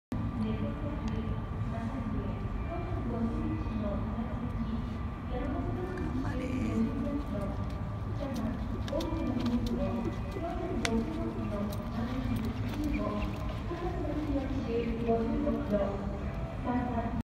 2018.12.11 船橋🐎1R サンアース&高橋利幸騎手、トウカイプリーズ&横川尚央騎手 本馬場入場
サラ系C3（二）1200m

7 サンアース（牡6歳）465kg（−9kg）8頭立て4番人気5着
高橋利幸騎手 函館一昭厩舎

5 トウカイプリーズ（牡8歳）477kg（+18kg）8頭立て6番人気4着
横川尚央騎手 宮下靖旨厩舎